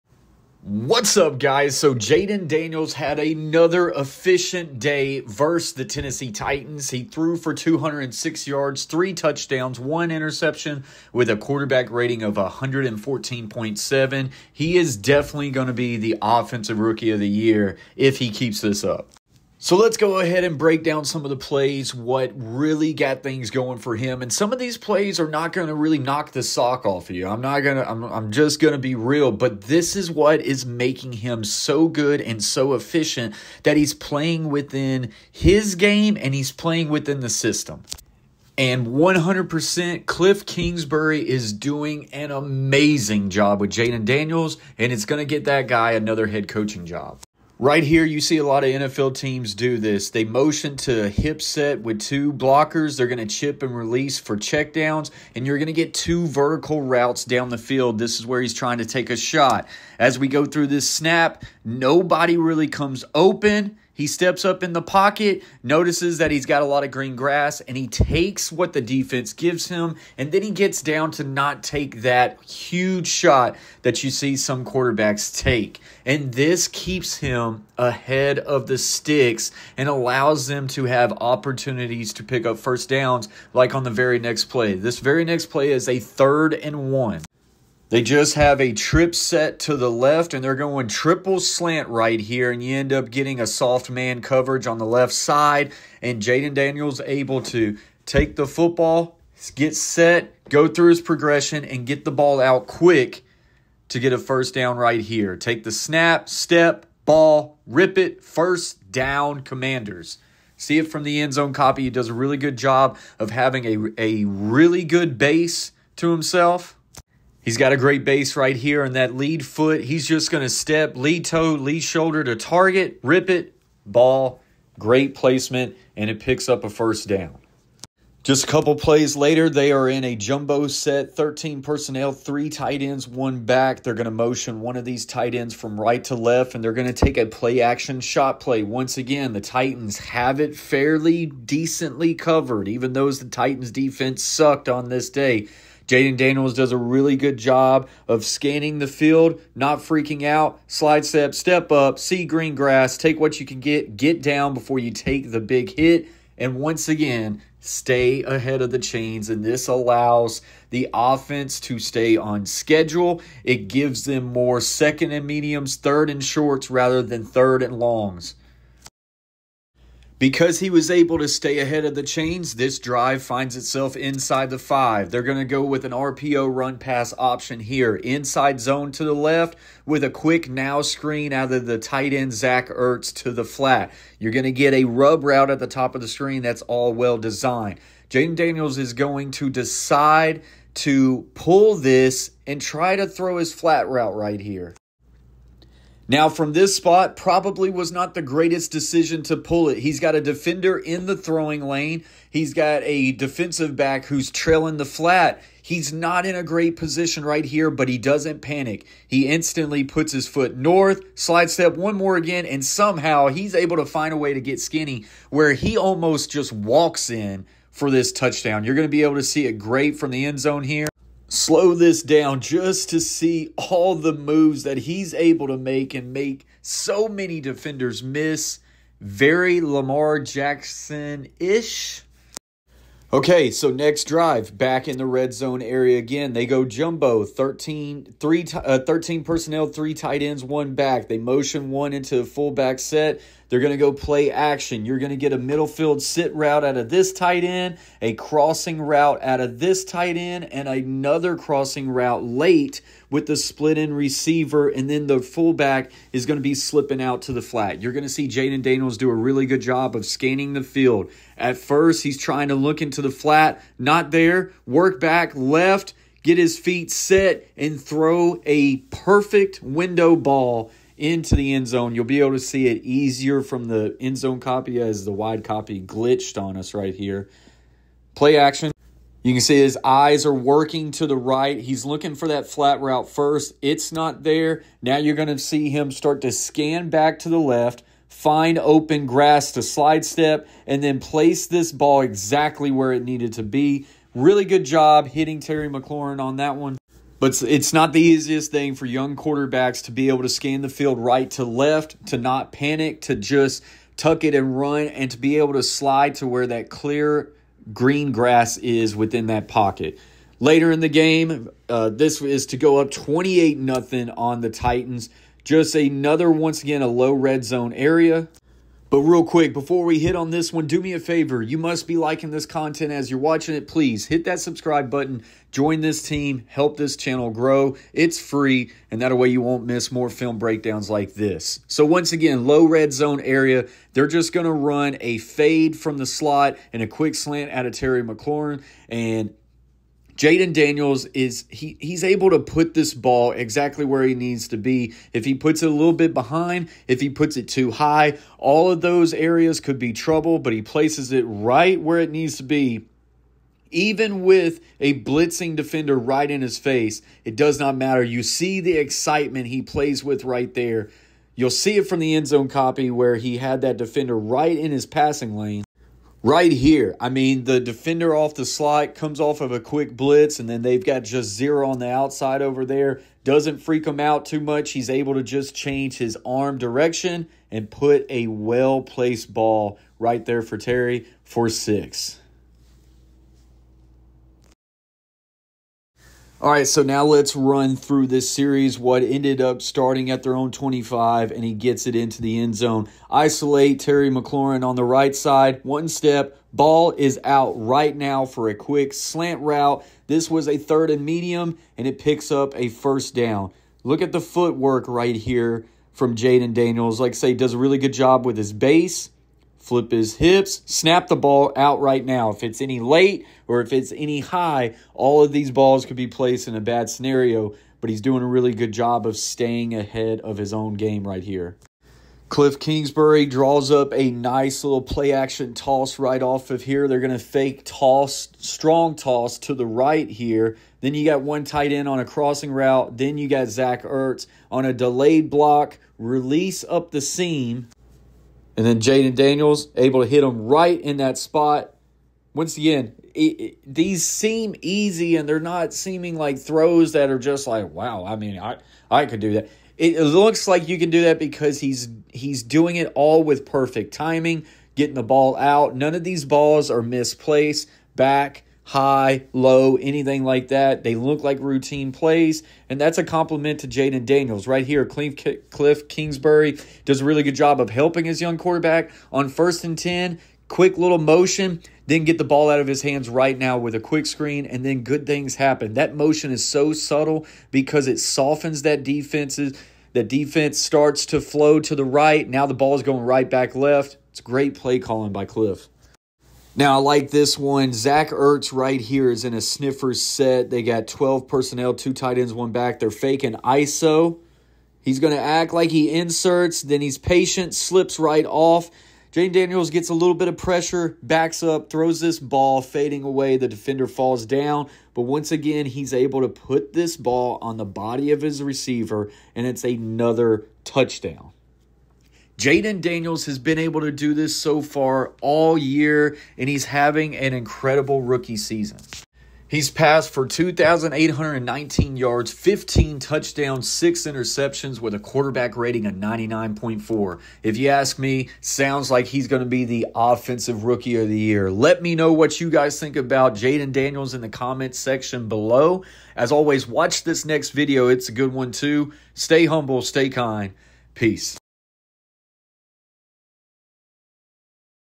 Breaking Down What Makes Jayden Daniels The Rookie Of The Year!
In this video, we break down Jaden Daniel‘s performance verse the Tennessee Titans, where he had a very efficient day throwing the football. Jaden Daniels has shown that he’s one of the best young rookies in the NFL and he keeps performing at a high level every single week and he’s got the commanders playing really good football.
Let us know what you think about Jaden Daniel’s performance this past week and all season in the comment section below . Do you think he’s the offensive rookie of the year?